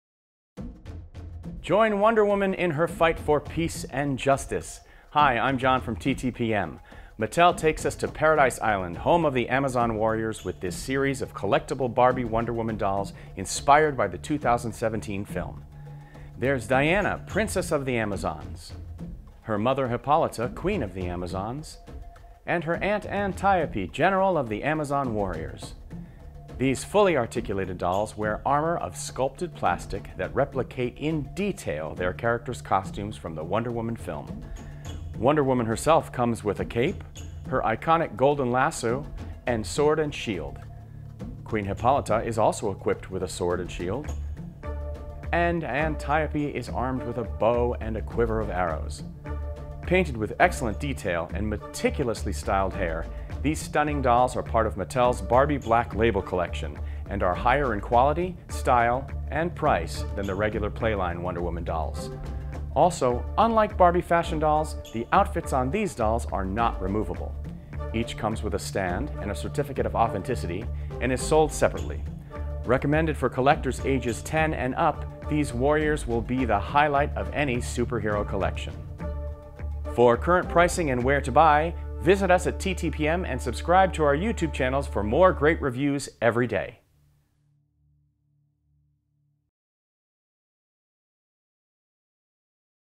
Barbie Collector Wonder Woman Hippolyta, Antiope & Wonder Woman from Mattel
This is a review for The Barbie Black Label Wonder Woman Collection Wonder Woman doll. For full review and shopping info

Product Info: Join Wonder Woman in her fight for peace and justice. Mattel takes us to Paradise Island, home of the Amazon Warriors with this series of collectible Barbie Wonder Woman dolls inspired by the 2017 film. This fully articulated doll wears armor of sculpted plastic and a cape that replicates in detail her character's costumes from the Wonder Woman film. Wonder Woman is armed with a sword and shield. Painted with excellent detail and meticulously styled hair, this stunning doll is part of Mattel's Barbie Black Label Collection and is higher in quality, style and price then the regular play line Wonder Woman dolls. Also unlike other Barbie fashion dolls, the outfit on this doll is not removable. Wonder Woman comes with a stand and a certificate of authenticity. Recommended for collectors ages 10 and up this warrior will be a highlight in any collection. Also available are Antiope, General of the Amazons and her sister Hippolyta, Queen of the Amazons. Each is sold separately.